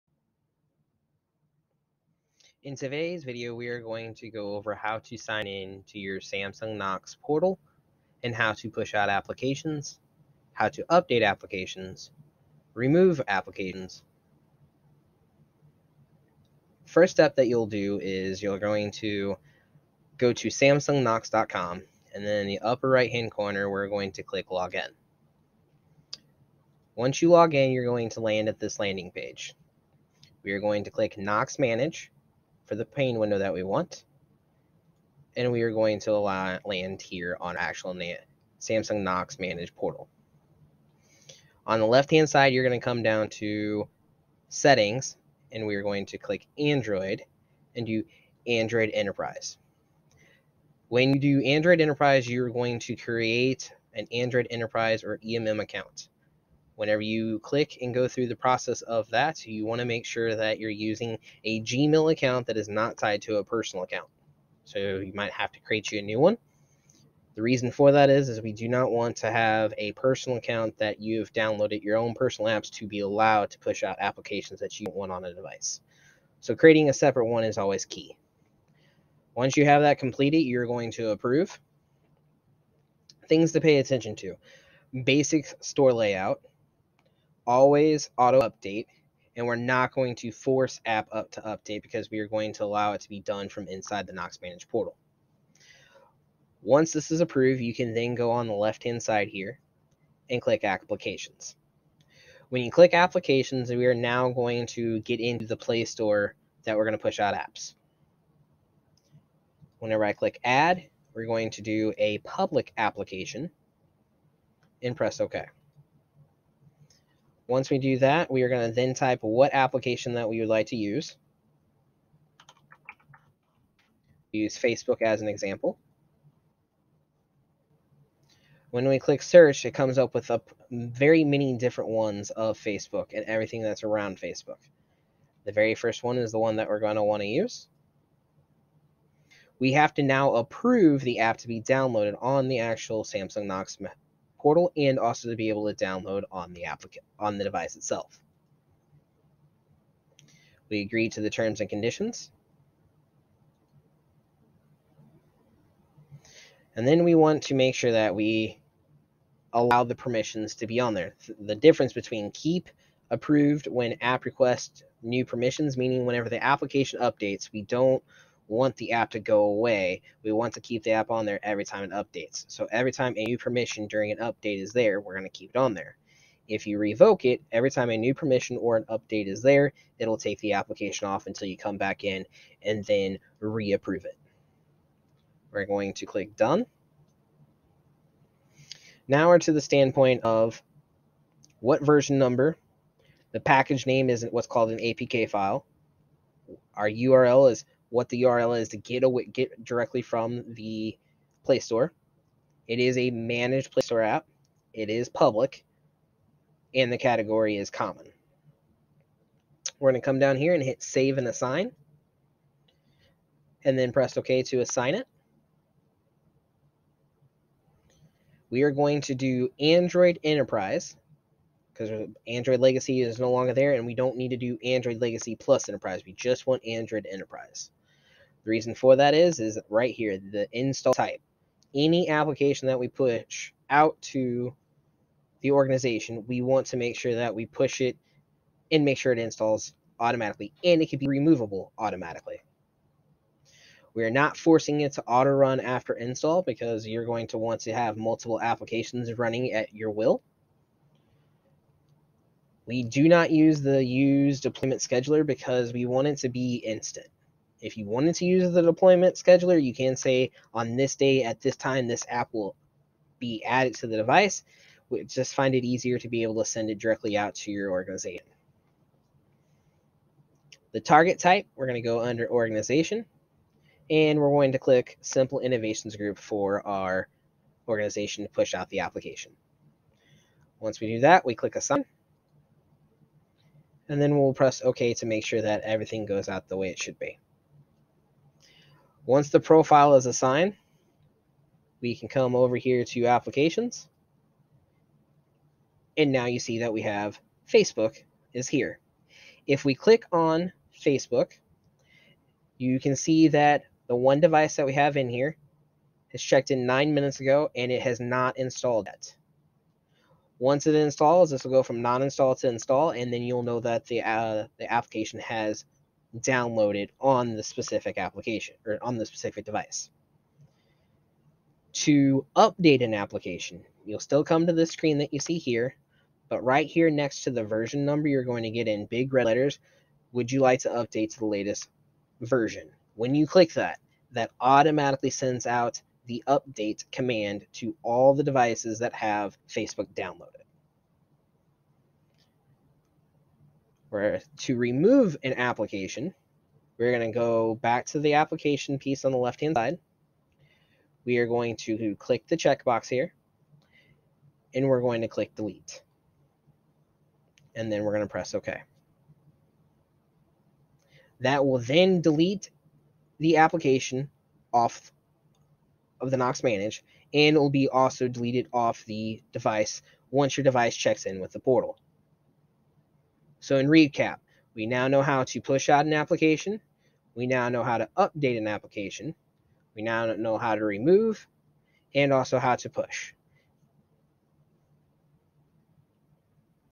Samsung Knox Manage Application Management
In this video tutorial, we cover application management for the Knox Manage portal. We show how to enroll your Android EMM account to add, update, push, and remove applications in the portal.

--- Timestamps ---
0:00 Intro
0:40 Android EMM Account
1:42 Add an Android Application
5:20 Update an Android Application
5:43 Remove an Android Application